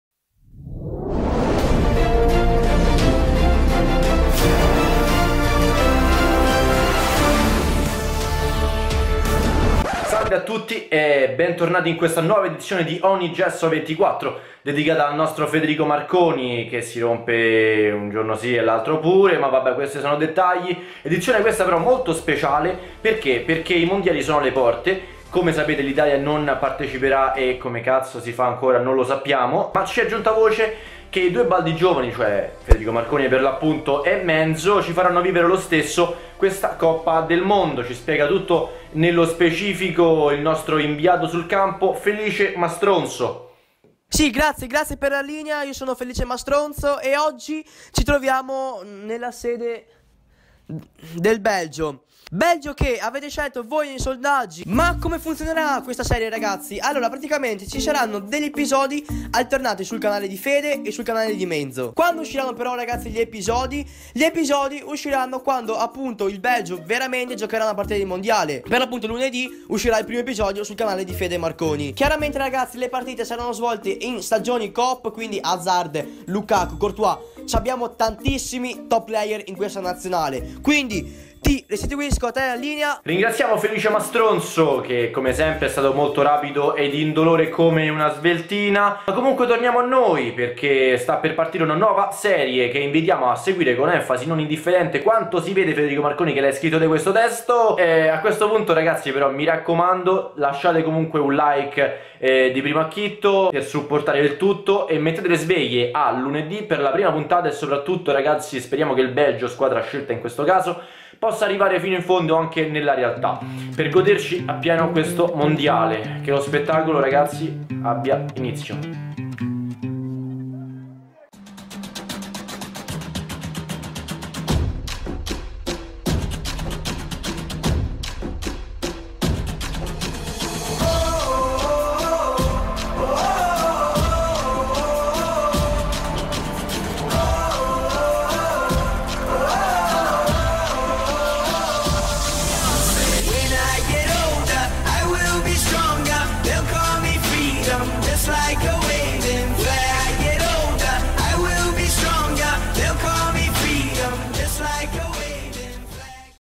ROAD TO WORLD CUP! [NUOVA SERIE] feat. Menzo
Inoltre un ringraziamento speciale a Edoardo Mongardini, per la "sigla promo" di questa serie!

👕 ACQUISTA la TUA MAGLIA e con MARCONI la SECONDA è GRATIS!

Come funziona la serie?
- io e Menzo giocheremo in co-op con il Belgio
- alterneremo un episodio sul mio canale e uno sul suo
- caricheremo ogni volta che giocherà il Belgio, quindi si parte LUNEDI' ALLE 14:30, qui SUL MIO CANALE!

Federico, 18.
📍Roma.